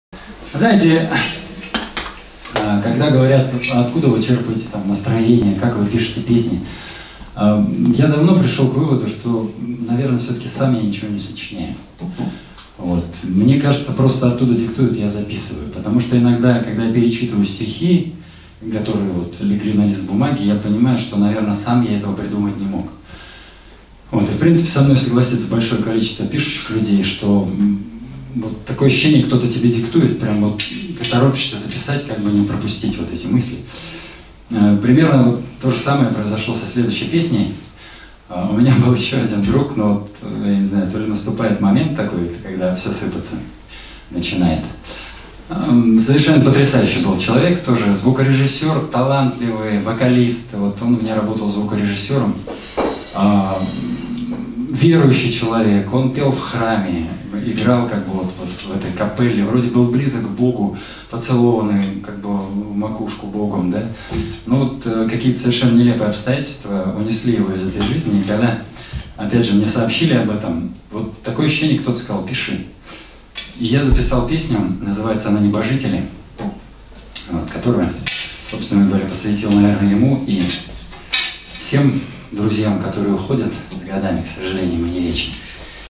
17 Рамиль Бадамшин
Рамиль Бадамшин на квартирнике Игоря Зорина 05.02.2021